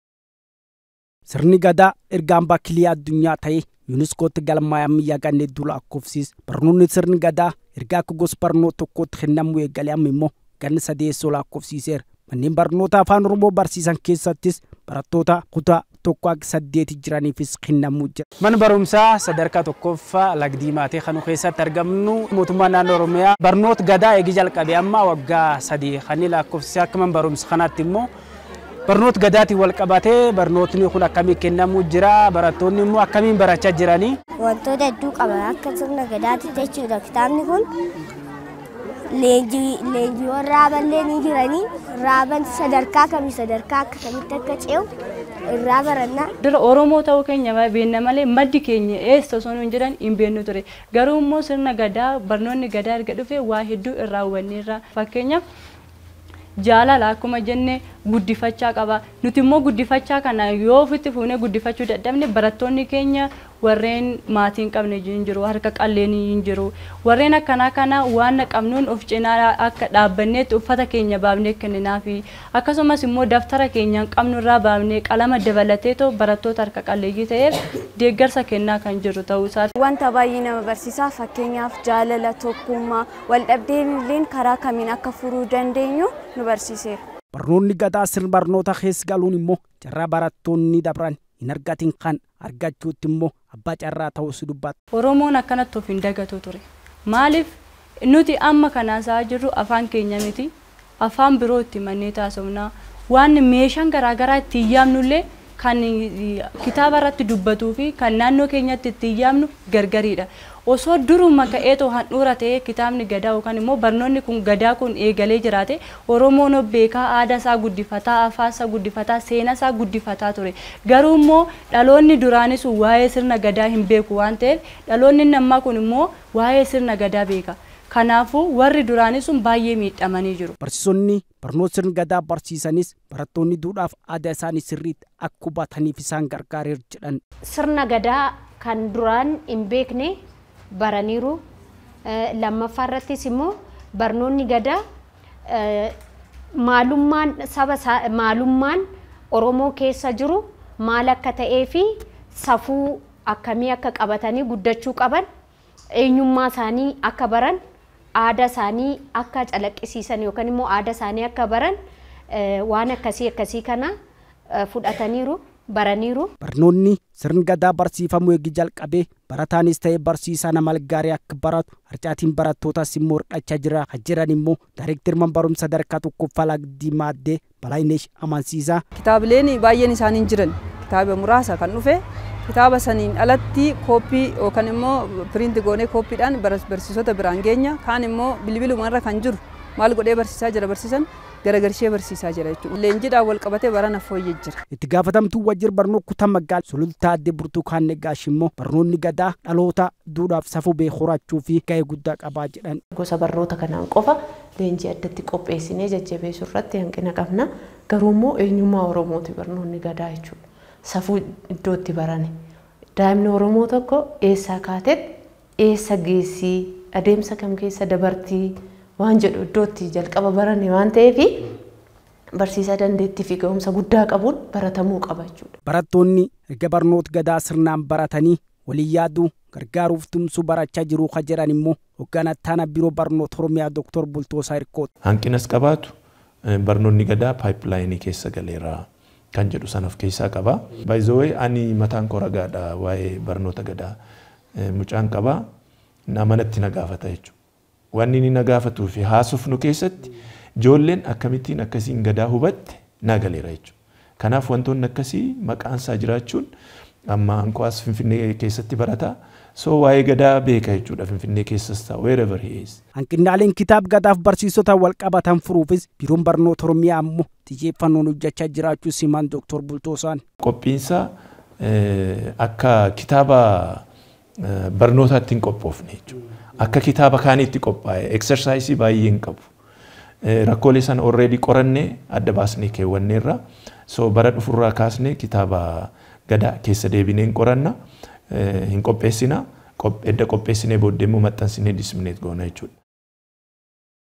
Barattoonni barnoonni Gadaa Safuu akka barannuuf nu gargaareeraa jedhan
Barattoonni Magaalaa Sulultaa barnoonni Gadaa eenyummaa, duudhaa fi Safuu keenya akka barannuuf nu gargaareera jedhan.

Barattoonni kunneen hanqinni Kitaabaa fi Barsiisaa akka furamuufis gaafatan.

Biiroon Barnootaa Oromiyaa Rakkoo Barsiisaa fi Kitaabaa jiru  furuuf hojjechaa jiraachuu beeksiseera.